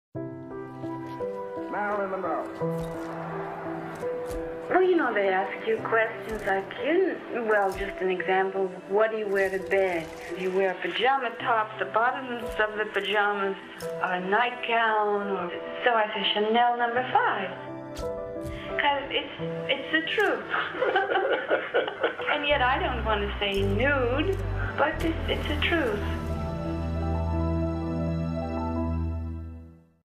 Chanel N°5 - Marilyn Monroe
Le Chaînon Manquant a recherché des images d'archives (photo et vidéo) de Marilyn Monroe, négocié ces images et le droit à l'image

(Production : FALABRACKS - Année : 2013)